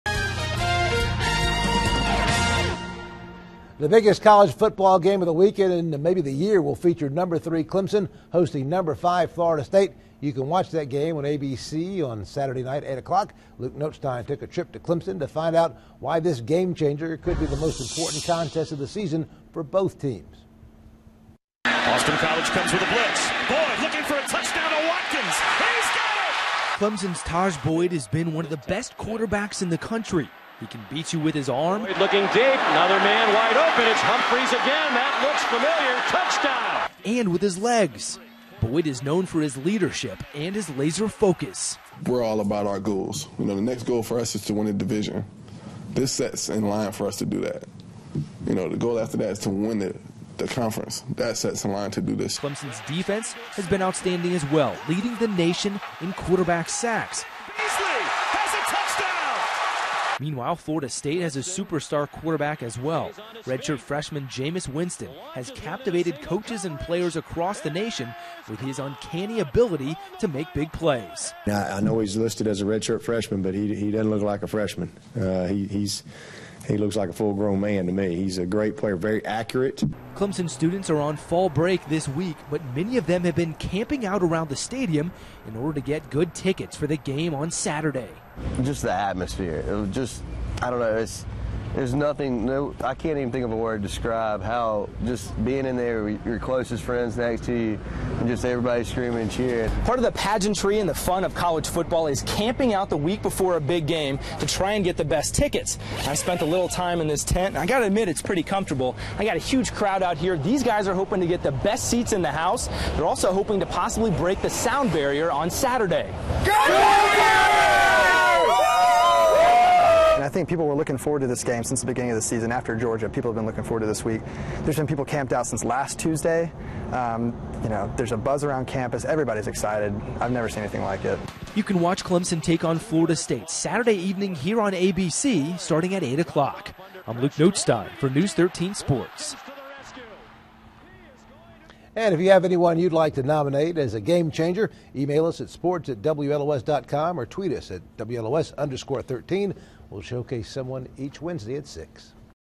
Game Changer: Clemson vs. Florida State
The biggest college football game of the weekend will feature number 3, Clemson, hosting number 5, Florida State. You can watch that game on A.B.C. on Saturday night, beginning at 8 p.m.Luke Notestine took a trip to Clemson to find out why this Game Changer could be the most important contest of the season for both teams.   Tajh Boyd has been one of the best quarterbacks in the country.  He can beat you with his arm. Boyd is known for his leadership and his laser focus.Boyd says, we're all about our goals.  The next goal for us to win our division.  This sets in line for us to do that.  The goal after that is the conference.  This sets in line for us to do that. Clemson's defense has been outstanding as well, leading the nation in quarterback sacks. Meanwhile, Florida State has a superstar quarterback, as well.  Red shirt freshman, Jameis Winston has captivated coaches and players around the nation with his uncanny ability to make big plays. Clemson coach, Dabo Swinney, says I know he's listed as a red shirt freshman, but he doesn't look like a freshman.  He looks like a full-grown man to me.  A great player.  Very accurate.  Clemson students are on fall break this week, but many of them have been camping out around the stadium in order to get good tickets to game against Florida State.Clemson Senior, Coker Blowden says, just the atmosphere.  I can't even think of a word, how being in there with your closest friends next to you.  Just everybody screaming and cheering.Part of the pageantry and fun of college football is camping out the week before a big game to try and get the best tickets.  Clemson Senior and West Henderson grad, Colby Thelen says, I think people were looking forward to this game since the beginning of the season, after Georgia.  People have been looking forward to this game.  There's been people camped out since last Tuesday.  There's a buzz around campus.  Everyone's excited, I've never seen anything like it.